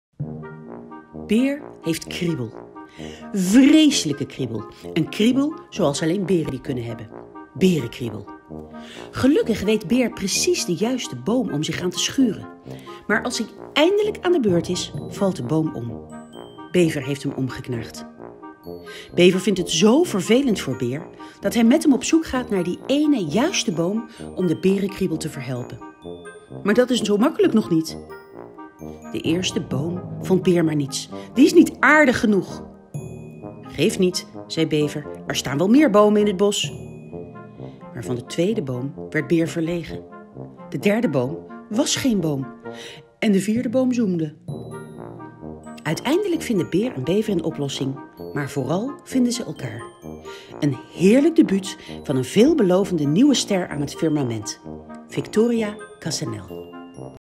Berenkriebel - Victoria Cassanell - Uitgeverij Lemniscaat
De aanbiedingstekst van Victoria Cassanell, Berenkriebel. Een prentenboek van Uitgeverij Lemniscaat. Bekijk ook eens